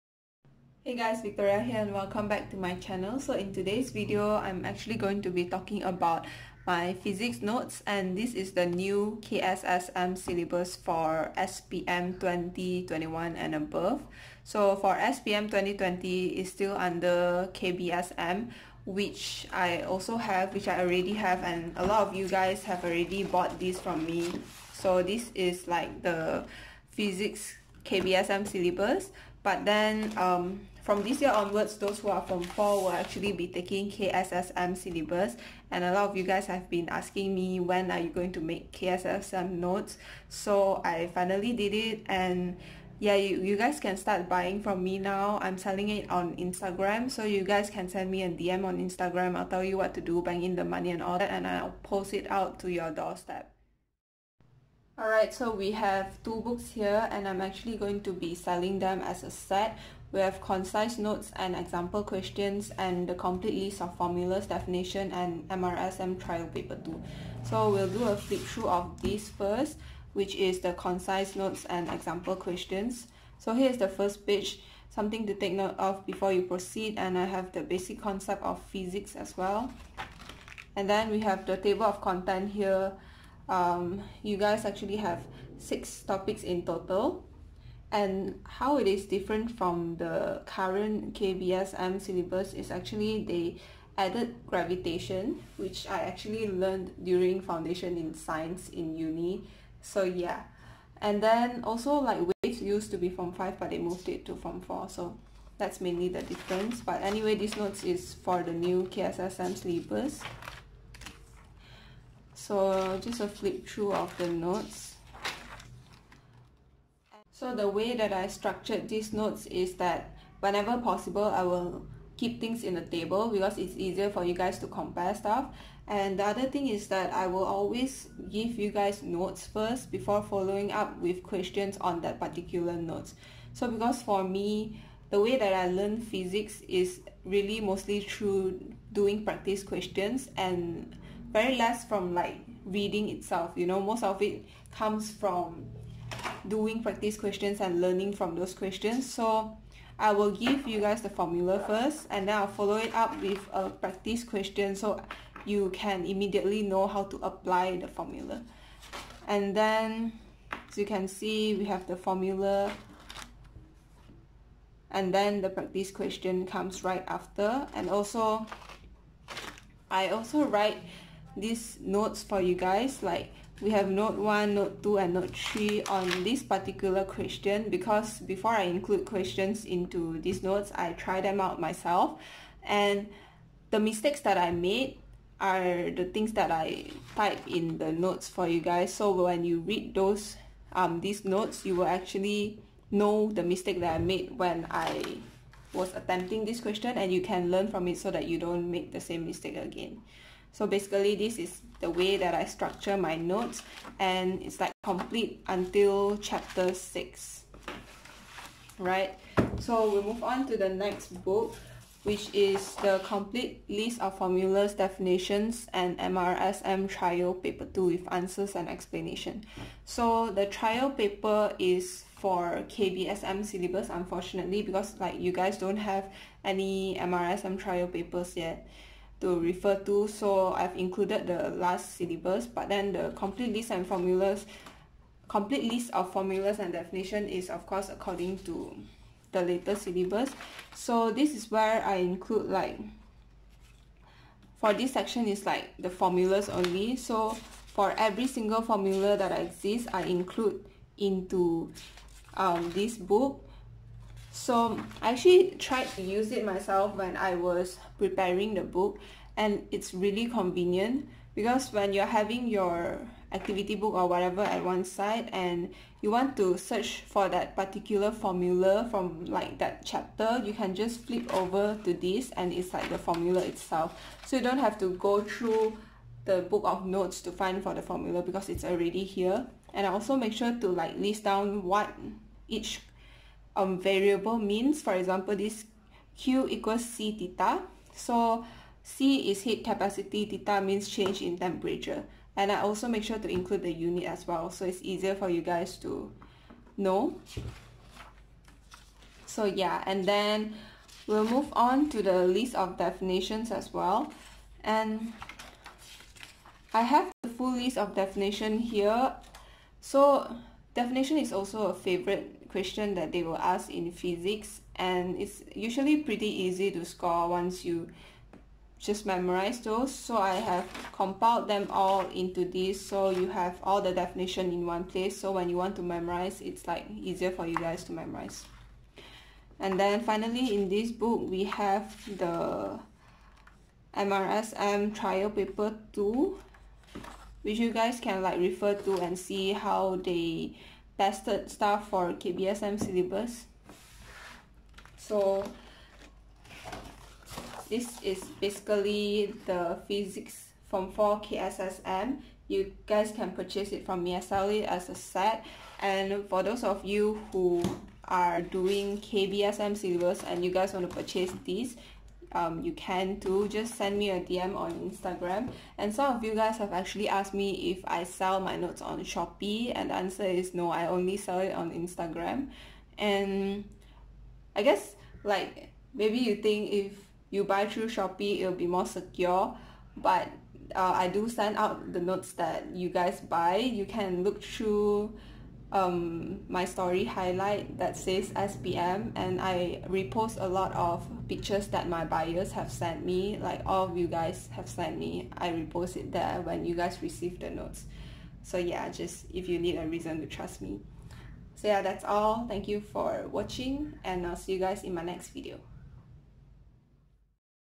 PHYSICS KSSM SPM NOTES FORM 4 + COMPLETE LIST OF FORMULAS AND DEFINITIONS | victoriactual
My Physics KSSM notes is finally out! Been working on these for so long and excited to launch it.

Head on over to my Instagram @victoriactual to purchase.